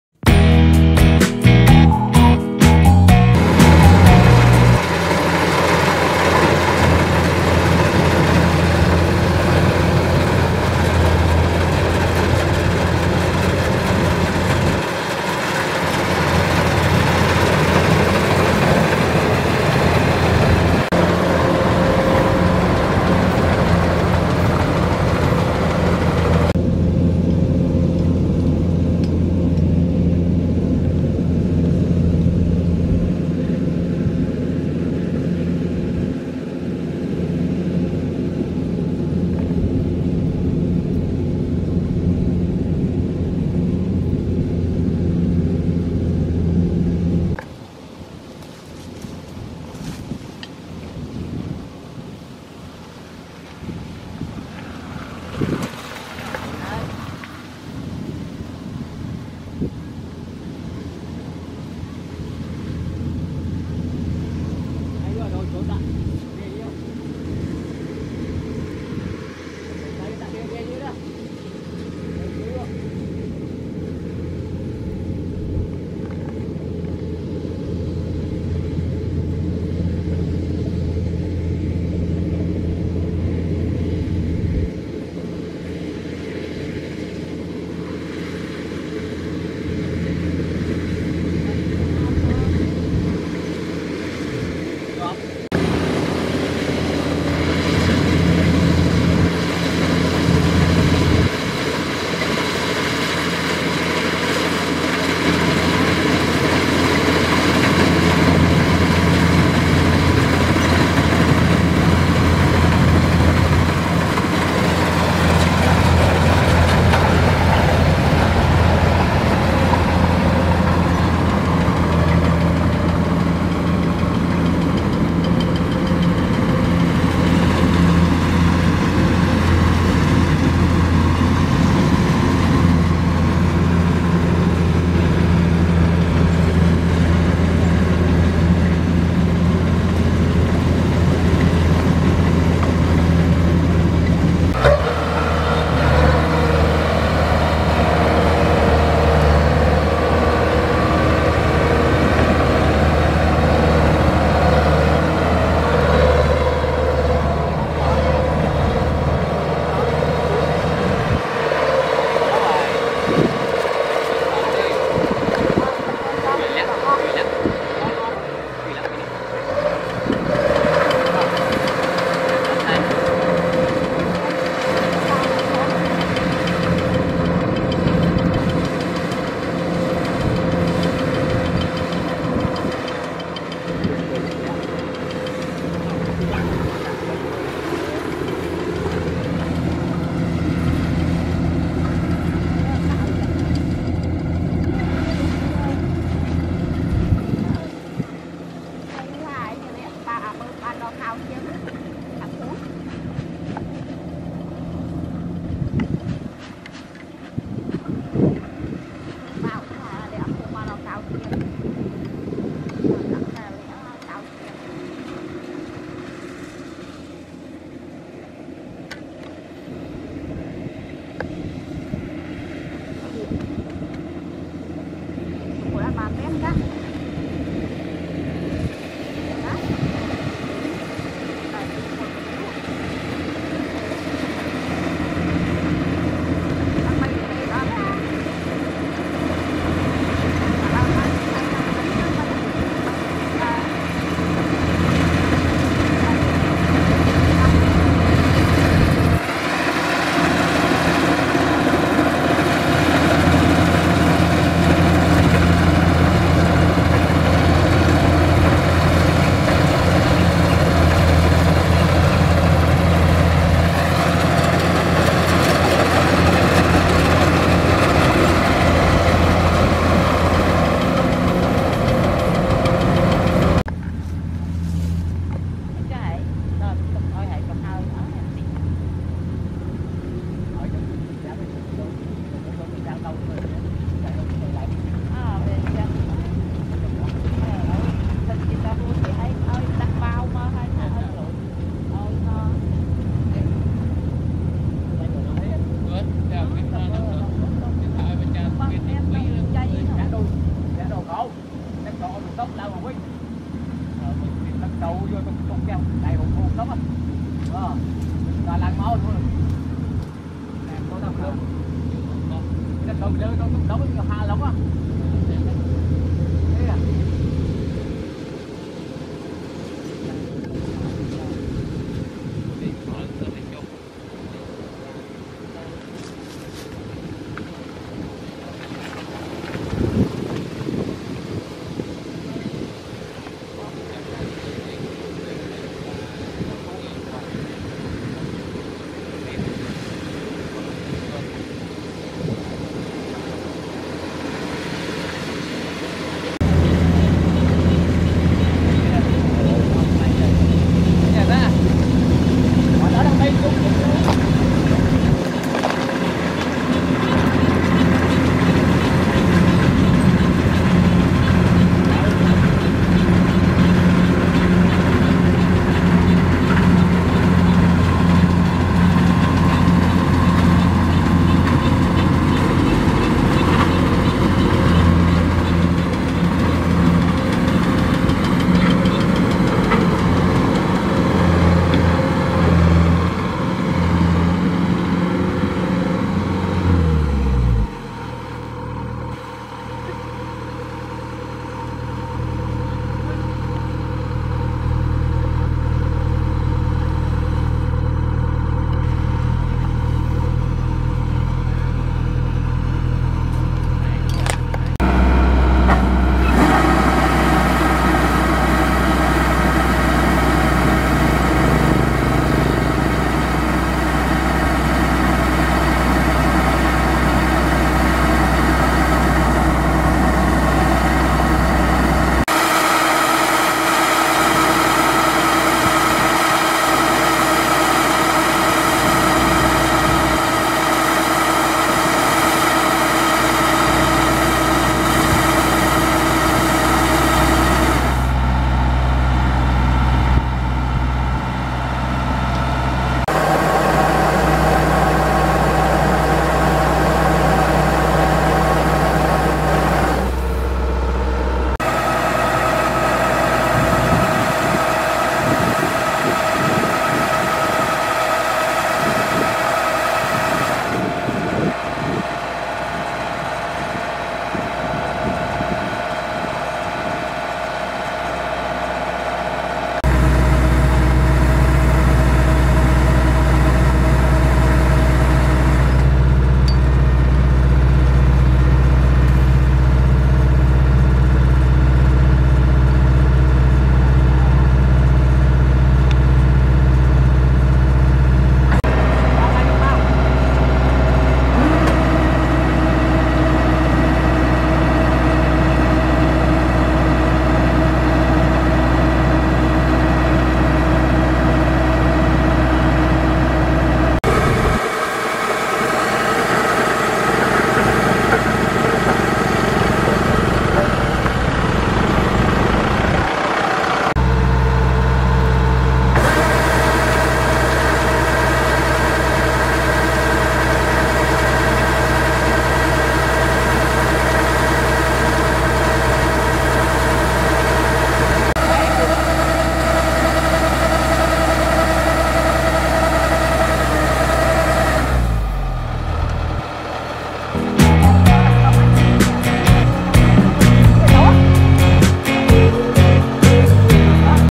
Harvester machine Kubota DC-105X Fast​ Cutting Rice- Agriculture crop machine |Daily agriculture
This video wants to show that the Machine harvesting Tractor KUBOTA in rice field. It has amazing harvested with the best powder of Machine harvester DC-105X on a person has an expert to using the machine for cutting rice. We will produce the goods the videos show about machine harvester and Machin pulling at rice field. It made in Cambodia.